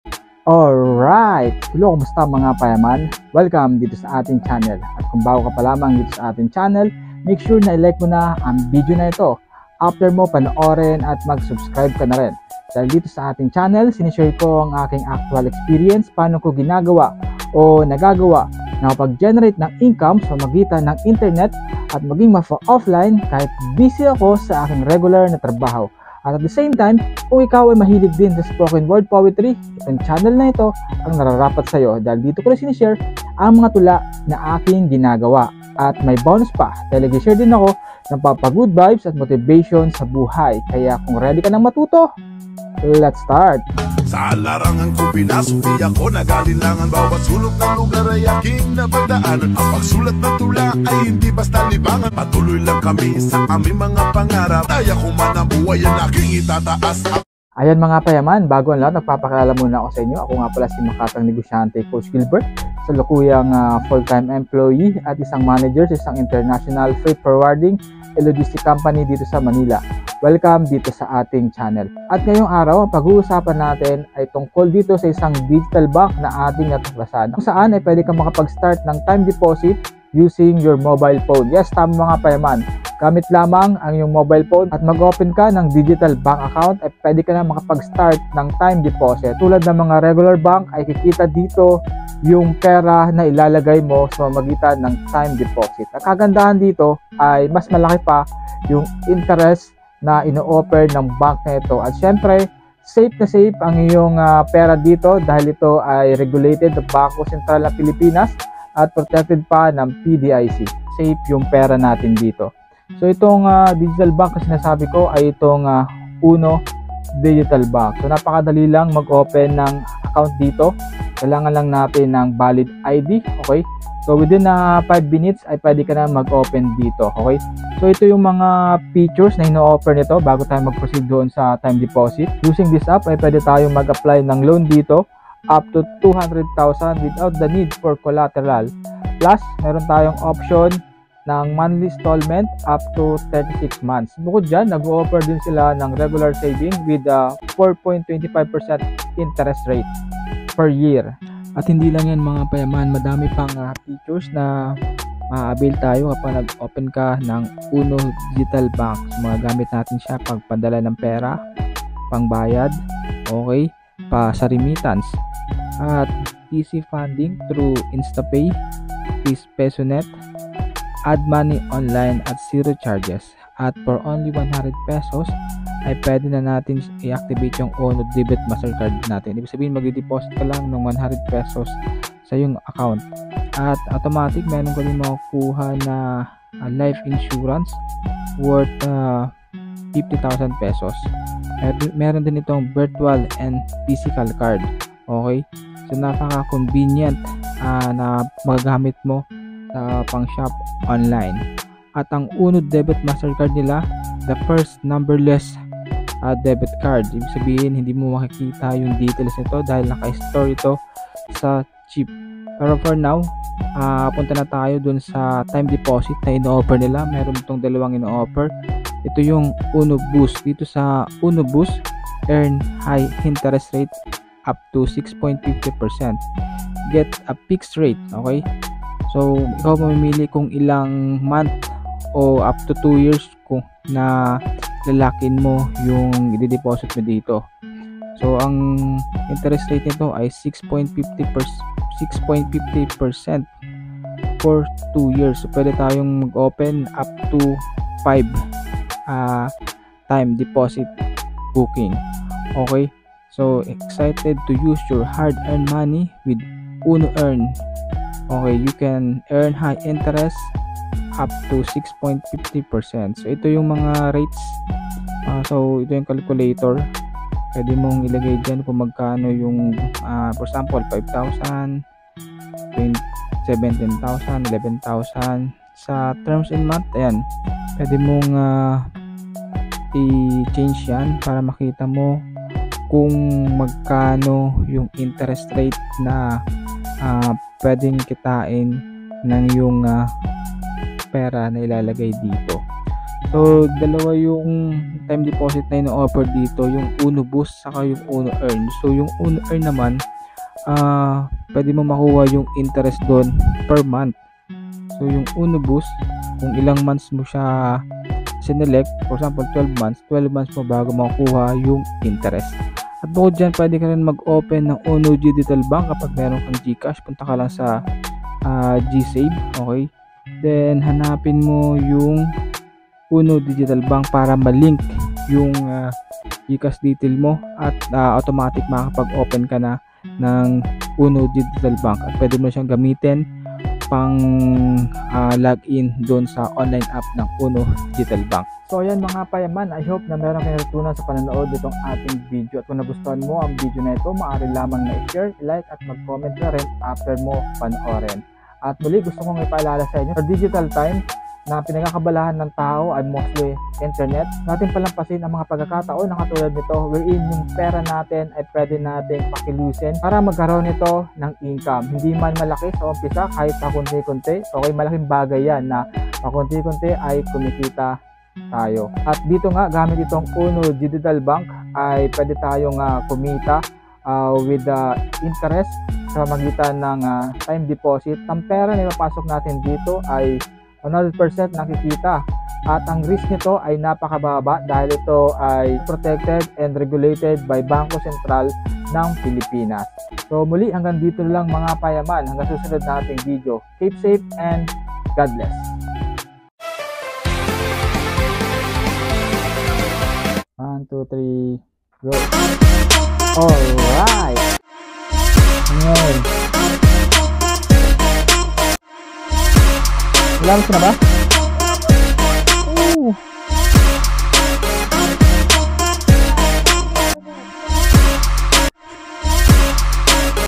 KUMITA SA TIME DEPOSIT GAMIT ANG CELL PHONE | UNO DIGITAL BANK
In this video at ishashare ko sa inyo itong DIGITAL BANK na ating nasubukan na kung saan ay pwedi kang kumita ng interest sa pag open ng TIME DEPOSIT, ito ay regulated ng bangko centra ng Pilipinas kaya siguradong safe ang iyong pera.

To open UNO DIGITAL BANK just download it your APP STORE or PLAY STORE,
or open via GCASH look gor GSAVE and select UNO DIGITAL BANK

Use referral number : MARKG62526

Maraming Salamat sa panonood and enjoy earning while using your money to work for you..